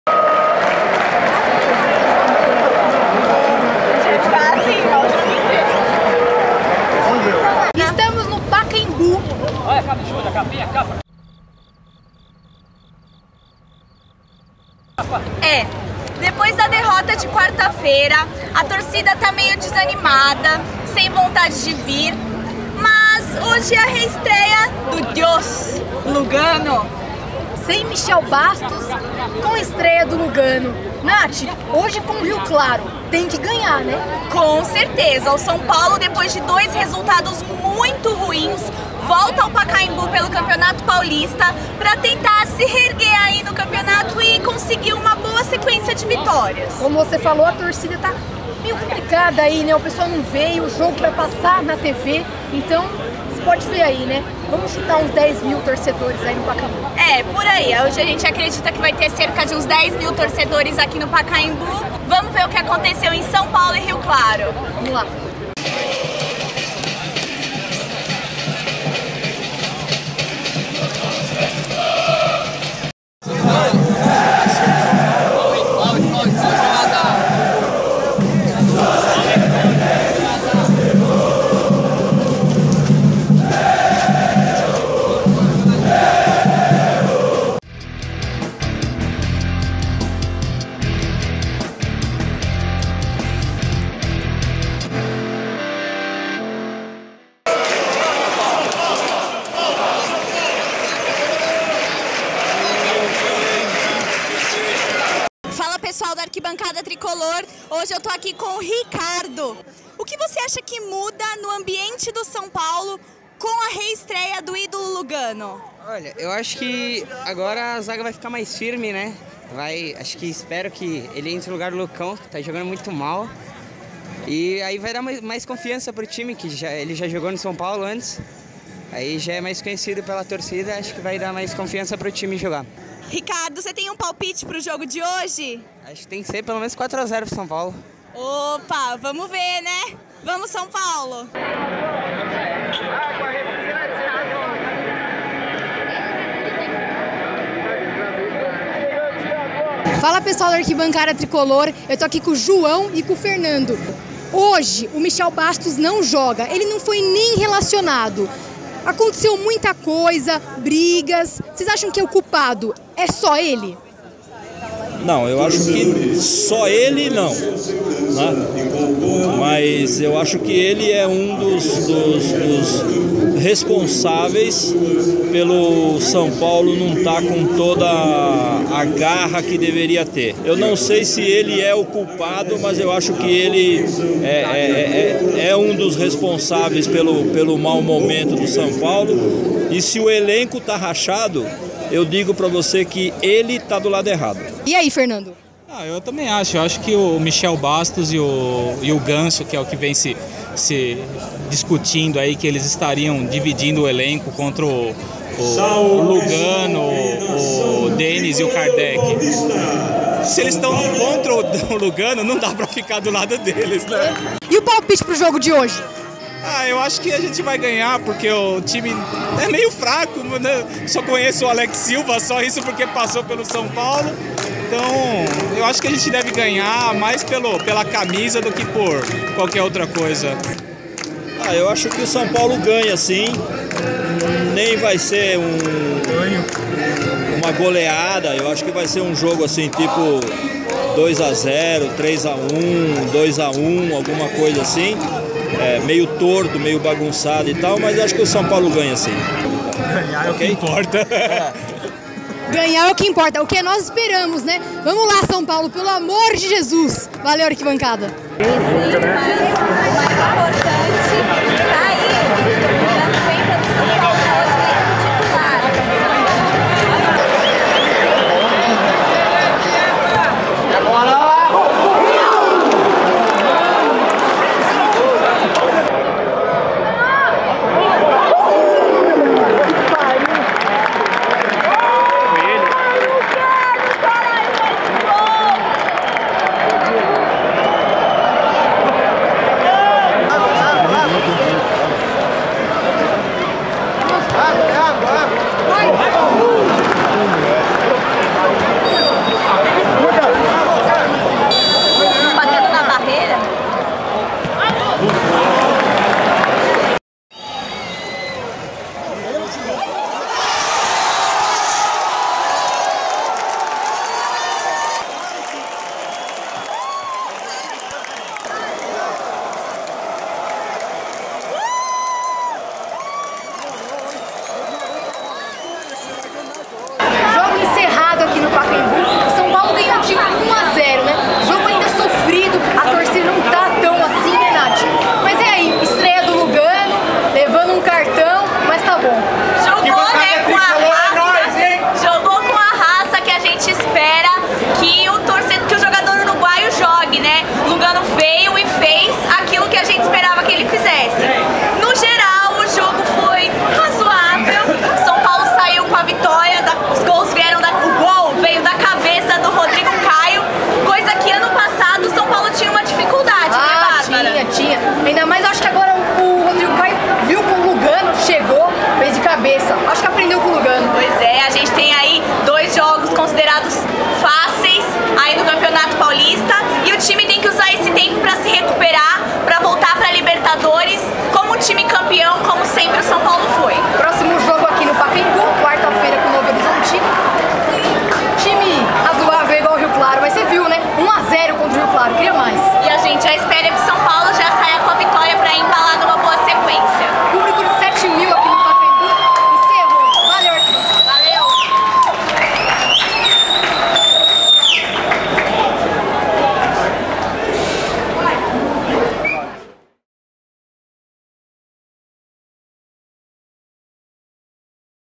Voz da Arquibancada - São Paulo 1x0 Rio Claro - Paulistão 2016
Em uma partida com várias chances de bola parada e pouca criação, o Tricolor venceu por 1x0 e esperamos uma semana mais tranquila. Houve protestos de torcida, estréia de Lugano e gol de Rodrigo Caio.